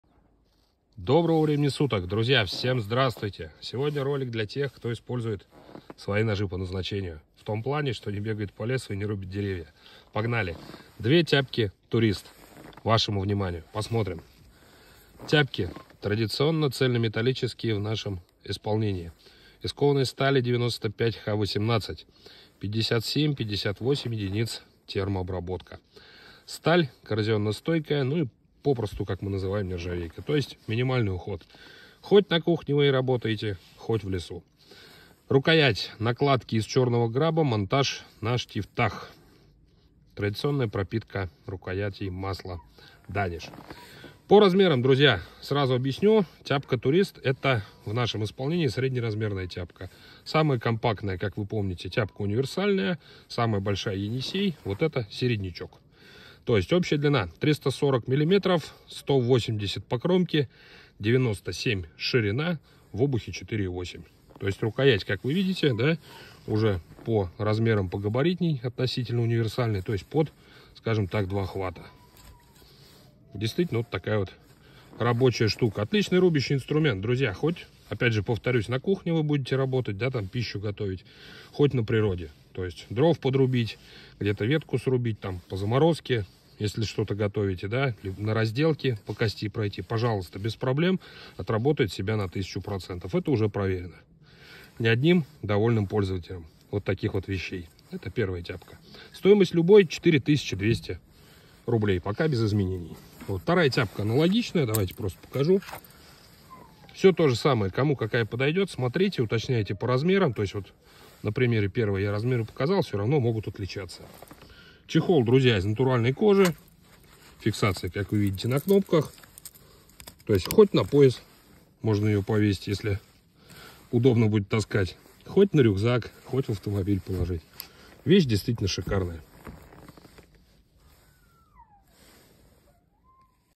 Рабочая тяпка «Турист»|95Х18 - 4200 рублей
______________________________________ •Клинок фуллтанг - кованая сталь 95Х18
ТО 57-58HRC
•Рукоять - накладки из черного граба(пропитка Danish oil)
•Ножны - кожа